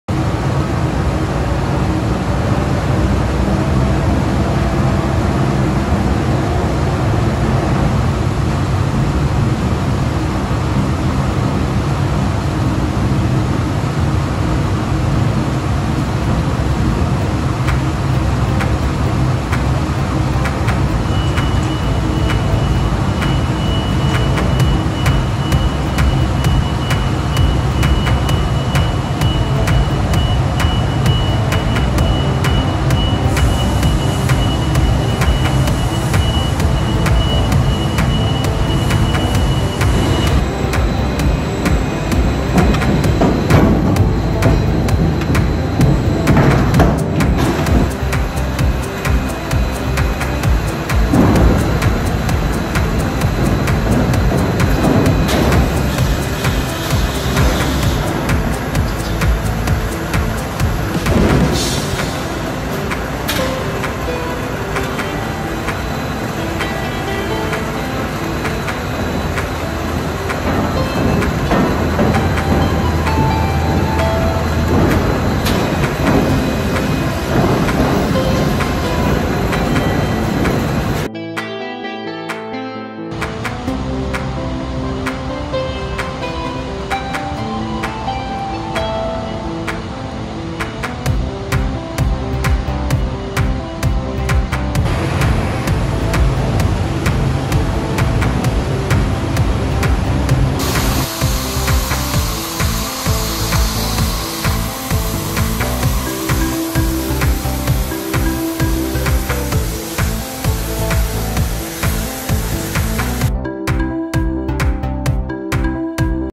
2020 03 24 we're Open and Rolling Steel with sound
We're open and producing top-quality plates and parts. Today we rolled ingots and slabs, flamecut multiple orders, conducted more than 10 UT tests, ran charpy and tensile tests through our in-house lab and planer milled pieces to exacting customer requirements.

If you don't want to take a chance on purchasing full plates for inventory in today's crazy market we will sell you pieces cut to your exact requirements.

We want to thank our team for pulling off a great production week so far. This video was recorded today from different production areas. Our team is doing its part to keep American manufacturing going strong. #